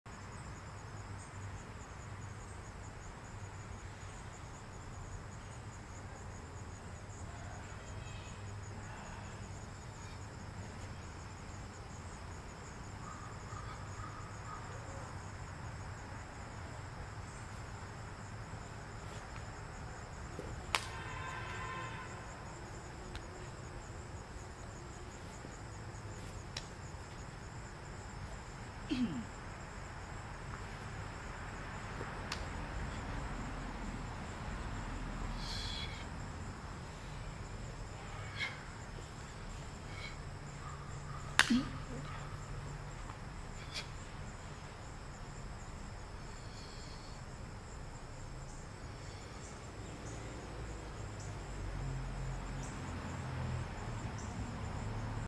Matsubayashi-ryu Rohai Kata
Working on Rohai Kata in my garden...